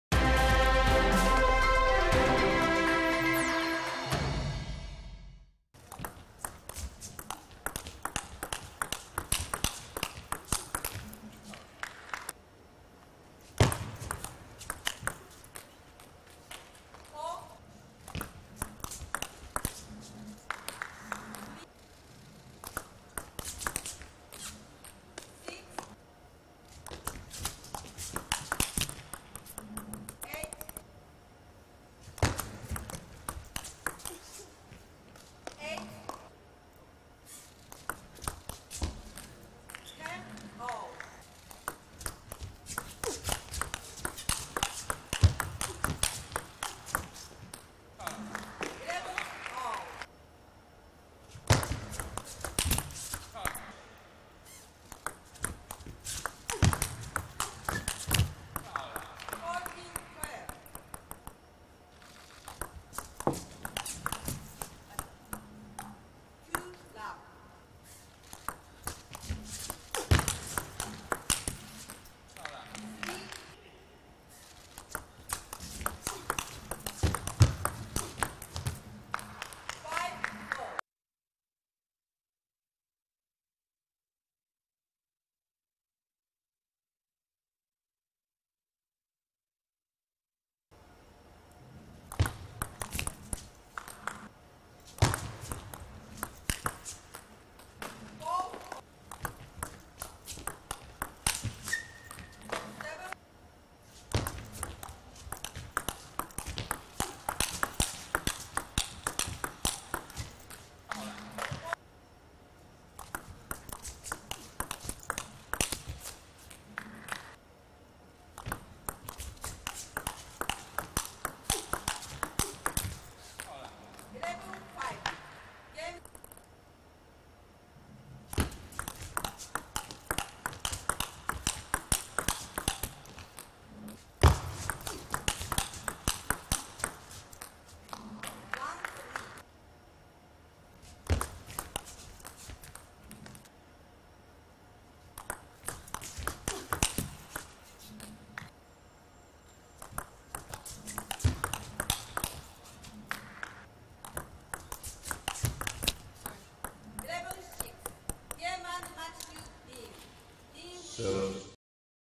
2016 World Tour Grand Finals Highlights: Ding Ning vs Lee Ho Ching (R16)
Review all the highlights from the Ding Ning vs Lee Ho Ching (R16) from the 2016 World Tour Grand Finals